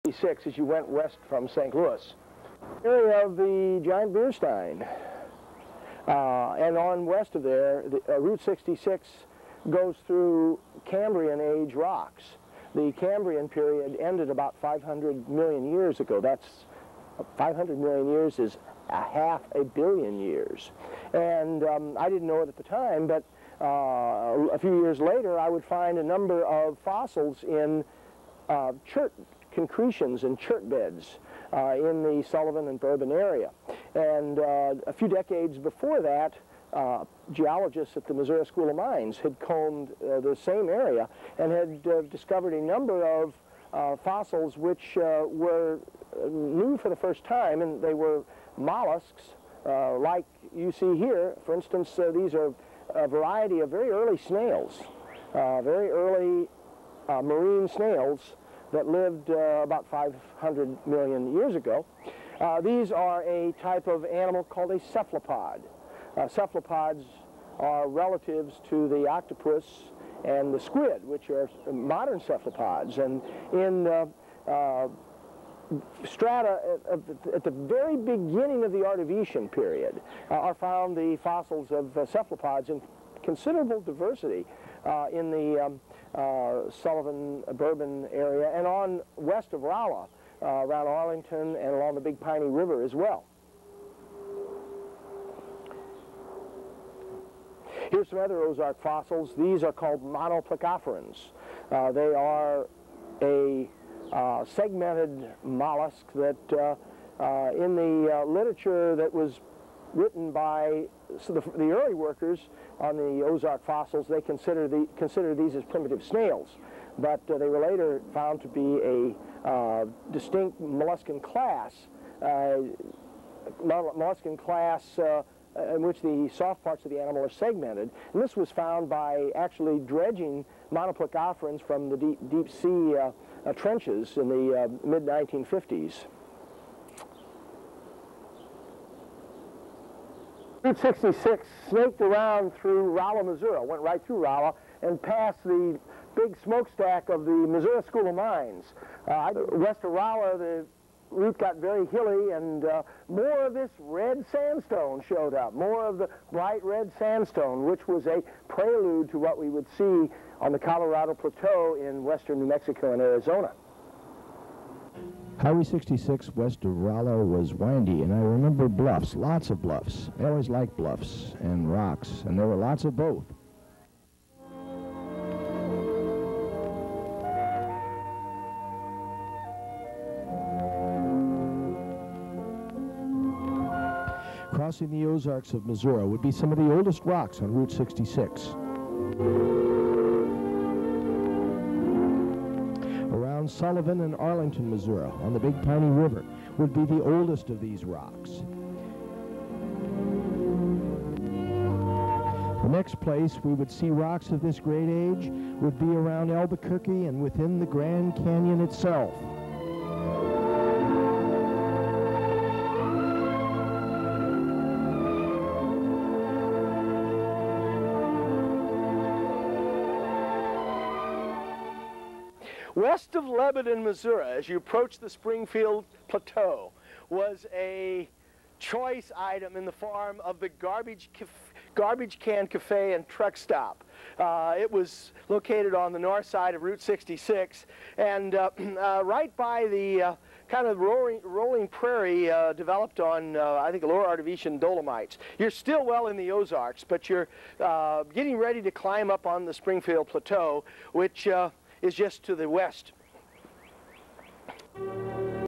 Stinchcomb, a geologic tour of route 66, crossing the Ozarks of Missouri
A geologic tour of route 66 which uses material taken (in part) from a trip done in 1950. Cambrian and Lower Ordovician fossils (and strata) are shown including primitive cephalopods, monoplacophorans and gastropods (snails).